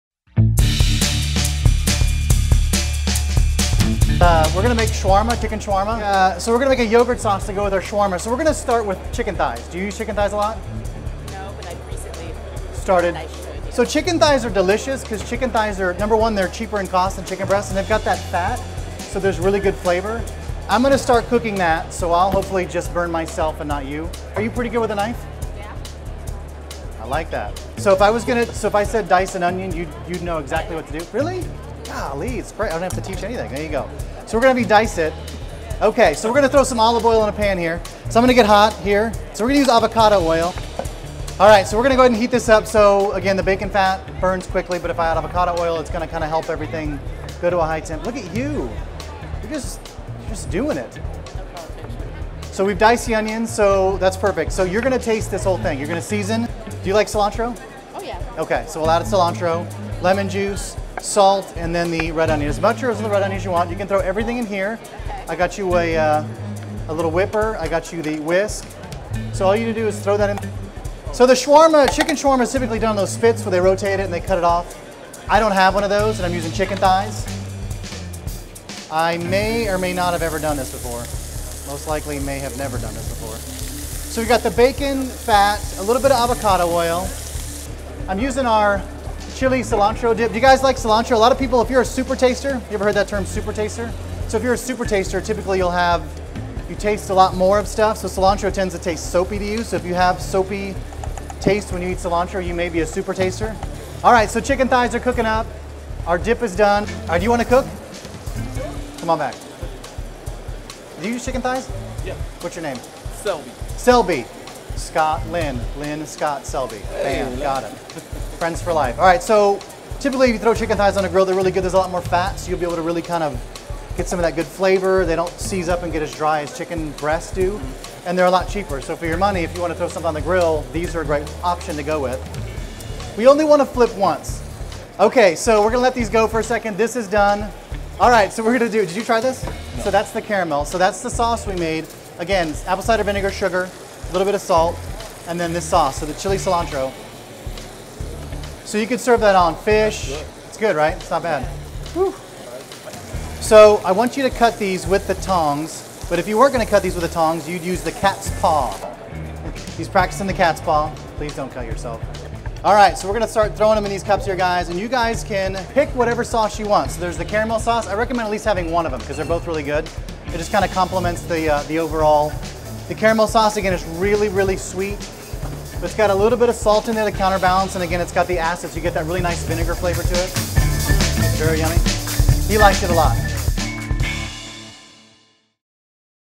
Everyday Gourmet Ep. 3 | Chicken Shawarma with Chili & Cilantro Dip
Chicken Shawarma is the latest foodie trend...get on board with our quick and easy recipe!

Chicken Shawarma with Chili & Cilantro Dip tastes just as exotic as it sounds. Arabic food is insanely flavorful just like our Chili & Cilantro Organic Dip. Try our ethnic spin on chicken wraps for an unbelievably good lunch or dinner.

Connect: @RobertRothschildFarm